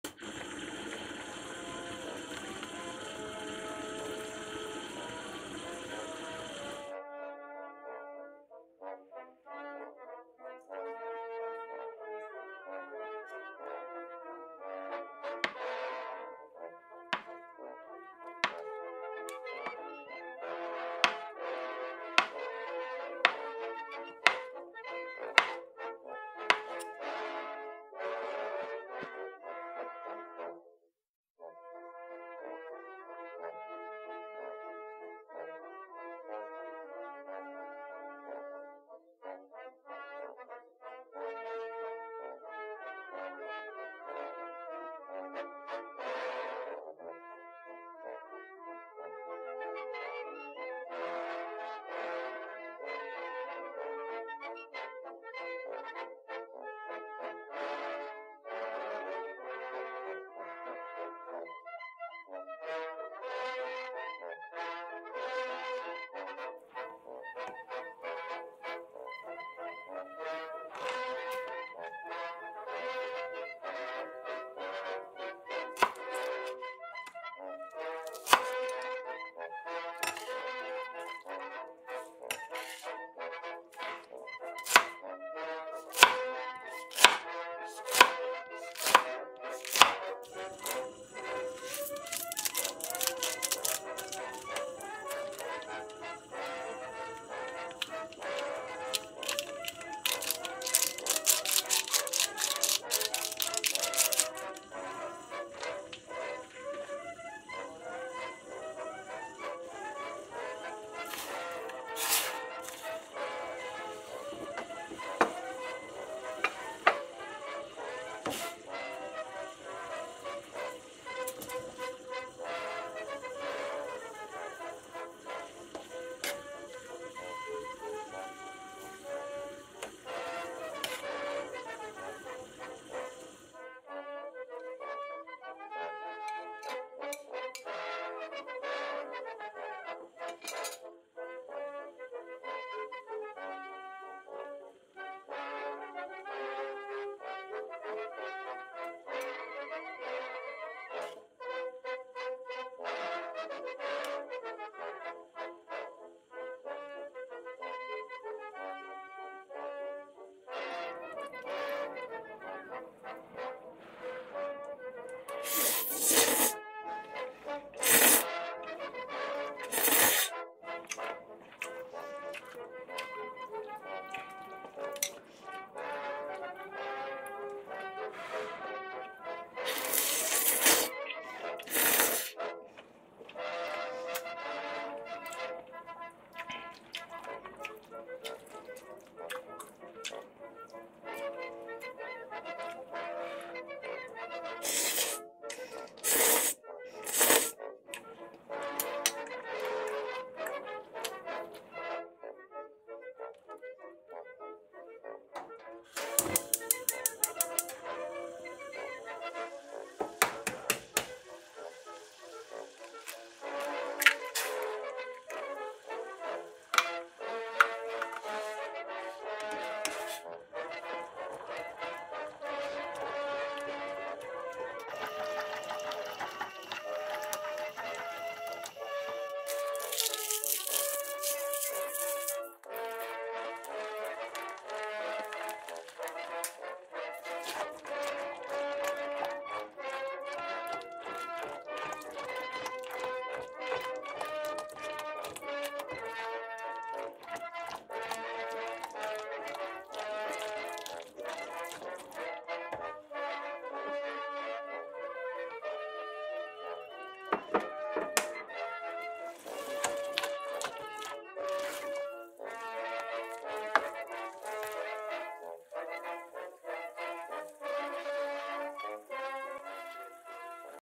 ENG 나혼자산다 어묵라면 with 라죽 만들기, 박나래표 레시피 | Fish Cake Ramen & Ramen Porridge Recipes
생각보다 훨씬 더 맛있어용!!
티비 보다가 급 먹고 싶어서 만들었어요